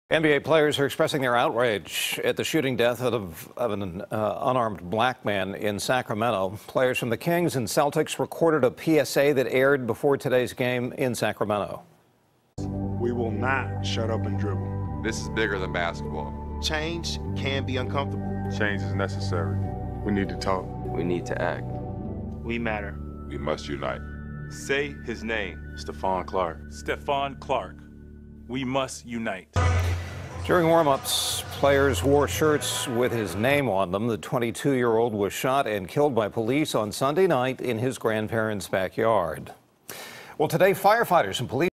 SACRAMENTO SHOOTING: NBA players issue PSA critical of Sacramento officer-involved shooting
NBA players issue PSA critical of Sacramento officer-involved shooting